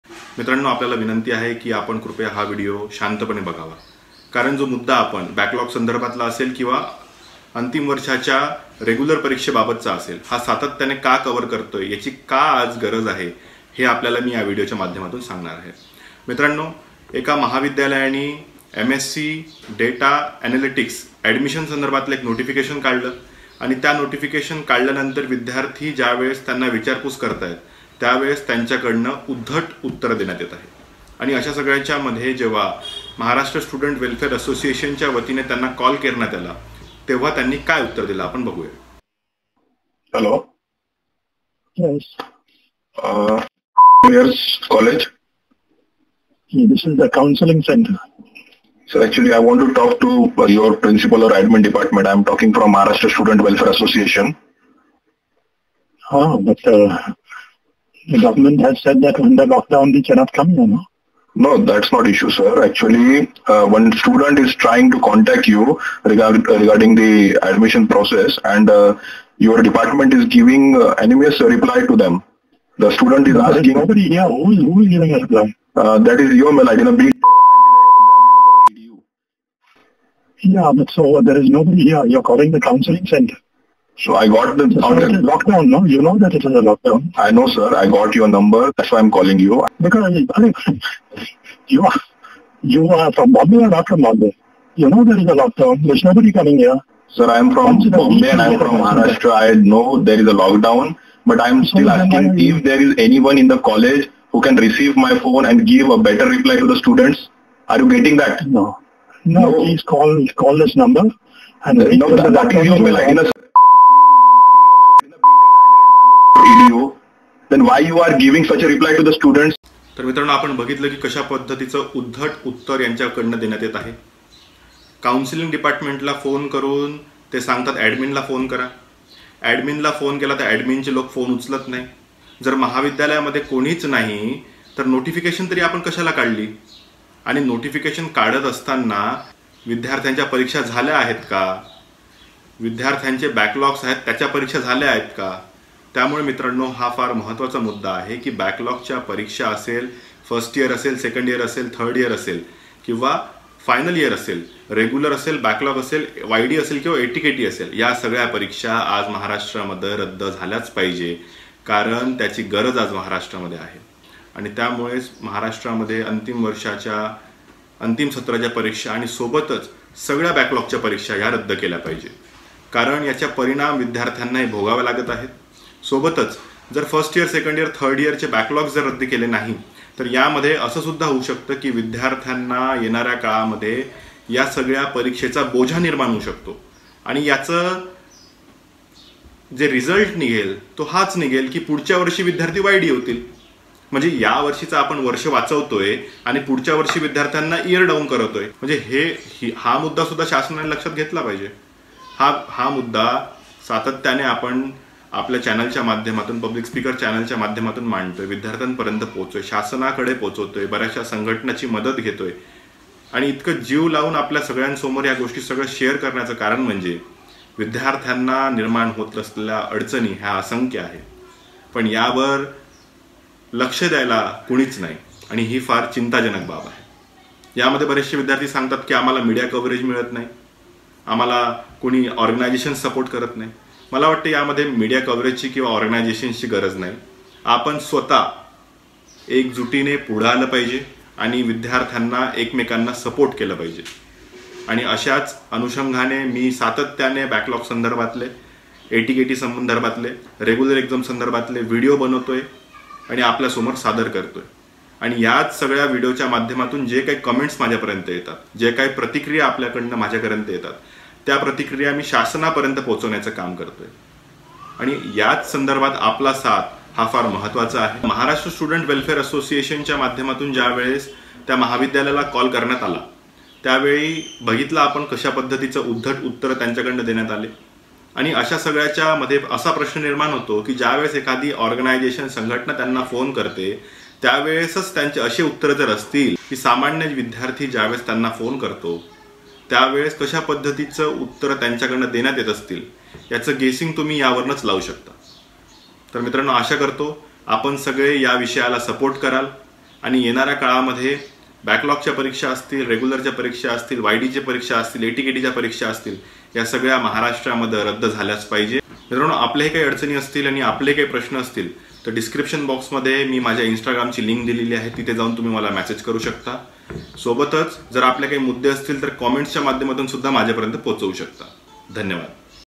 महाविद्यालयची मनमानी विद्यार्थ्यंना त्रास | SPPU BACKLOG | BACKLOG EXAM NEWS | ATKT
About This Video:
In this video we are going to see how backlog atkt Yd and final year students are facing problems due to exams